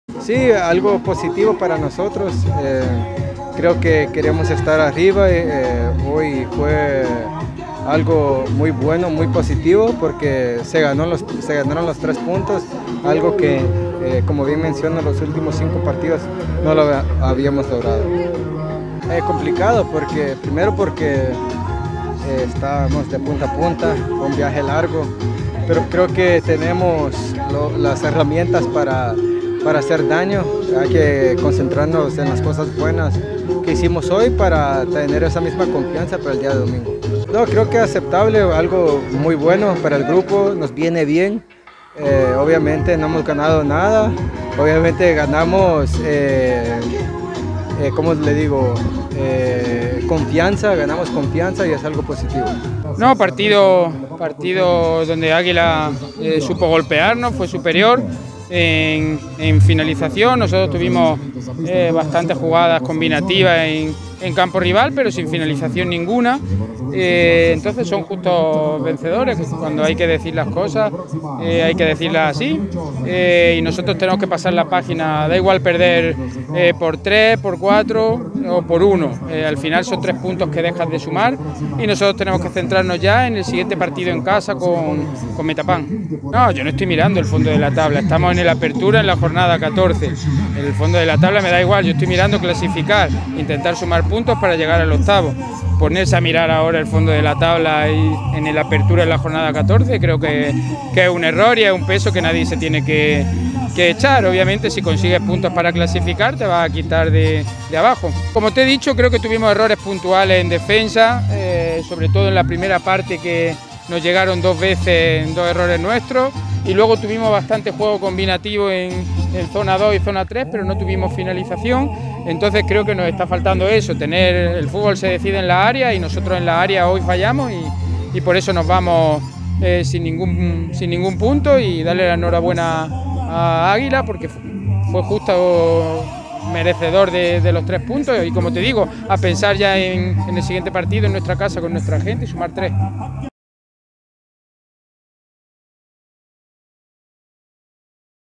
El Águila vence 4-0 al Platense en San Miguel
El cuadro emplumado se impuso por 4-0 al Platense. Esto dijeron después del partido.